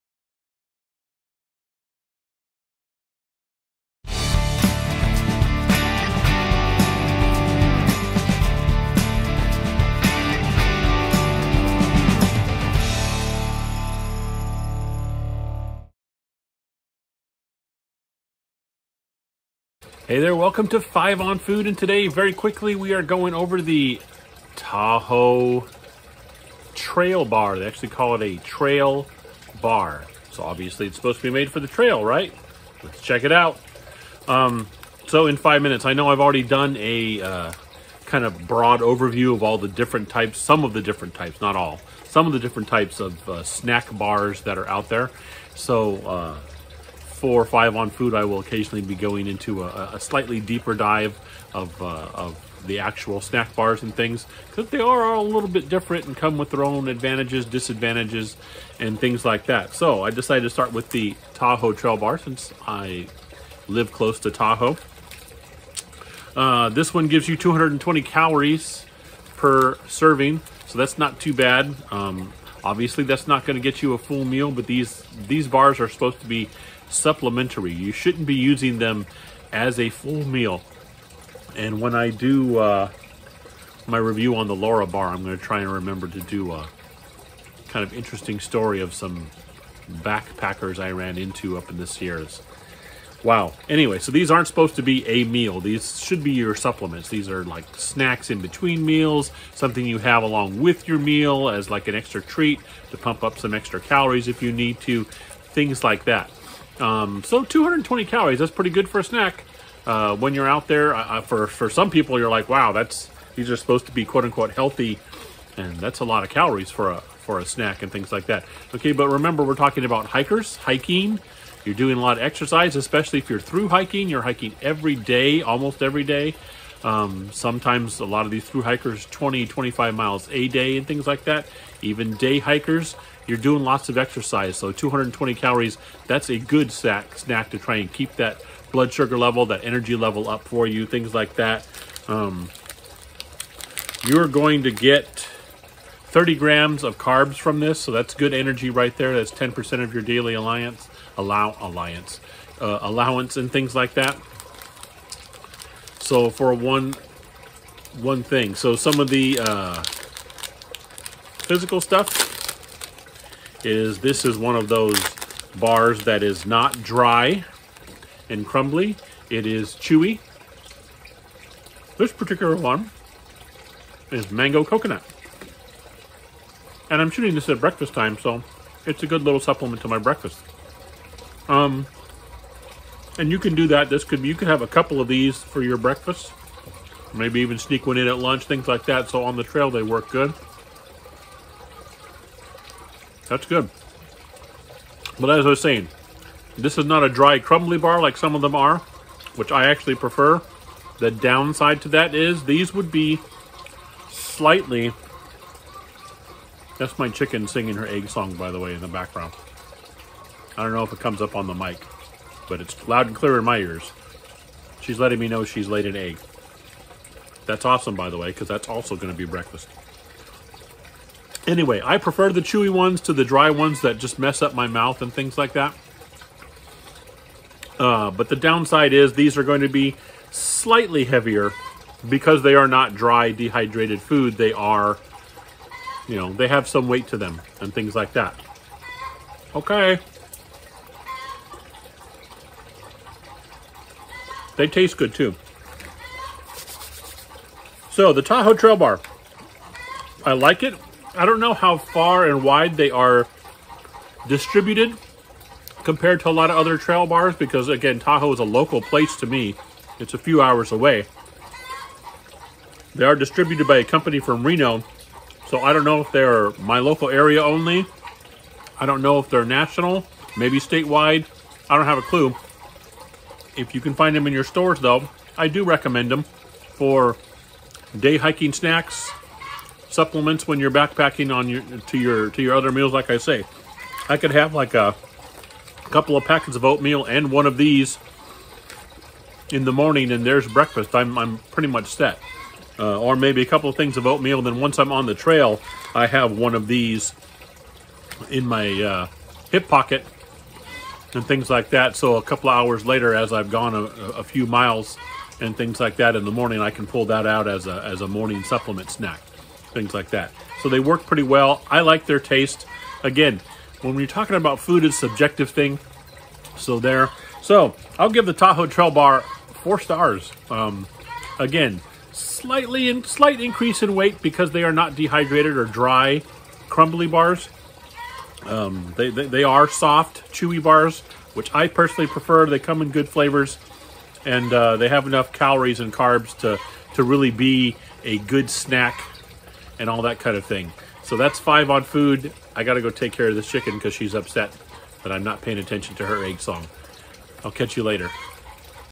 5 on Food: Tahoe Trailbar Review
Here's a quick look at the "Tahoe Trailbar" as far as its hiking uses.